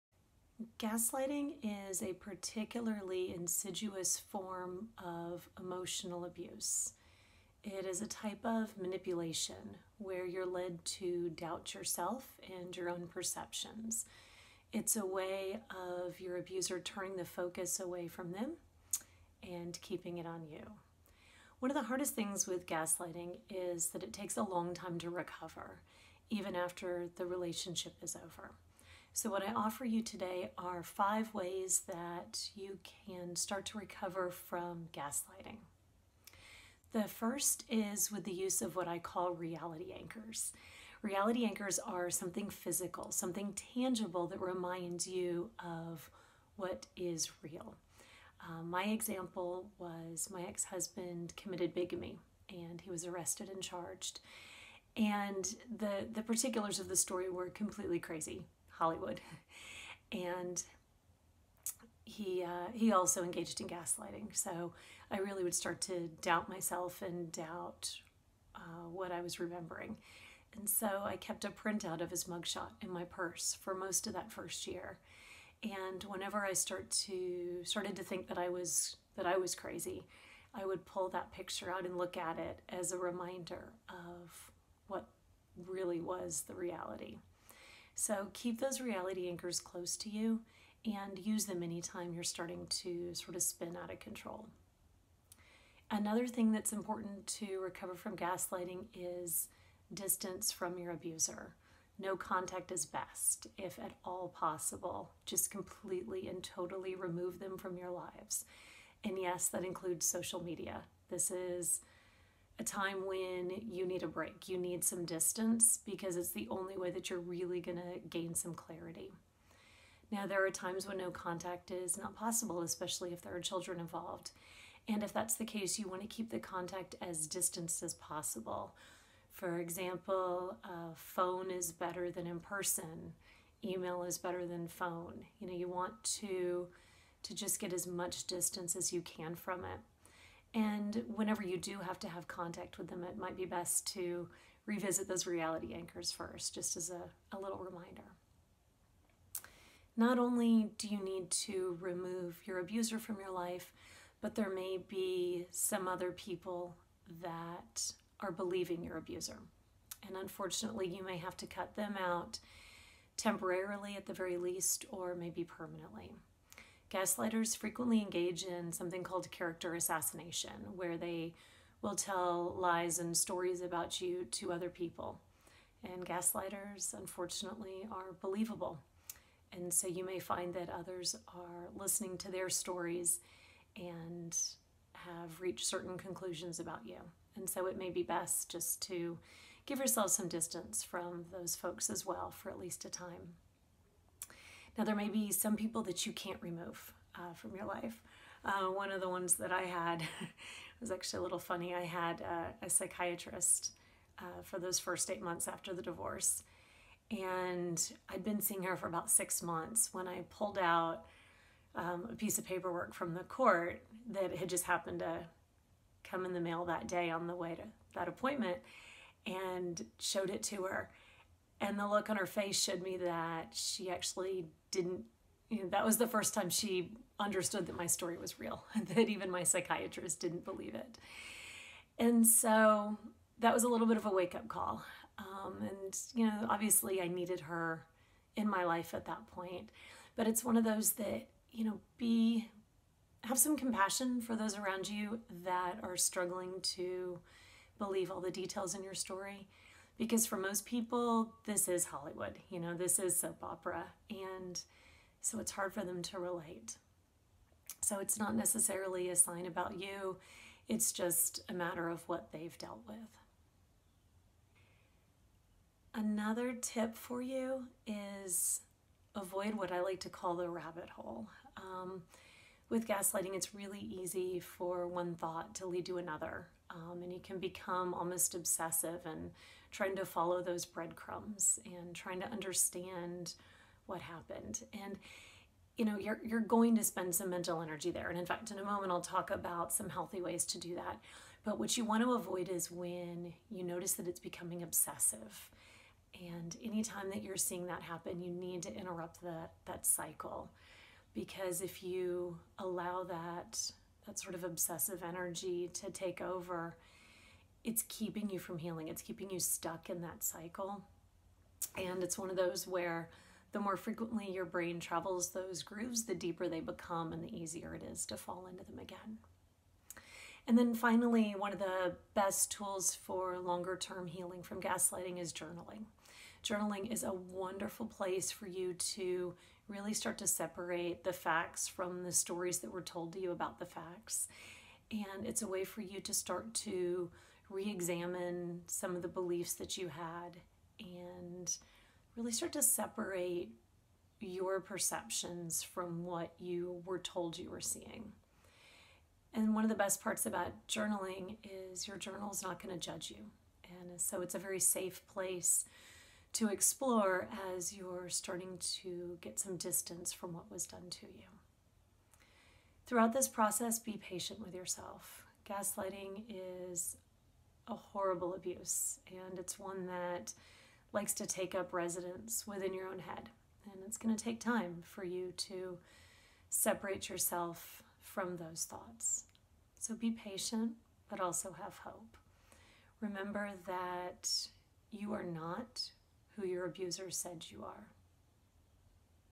Recovering From Gaslighting
Gaslighting is a relentless and crazy-making tactic used by narcissists and other covert abusers. Here are five strategies to help you recover.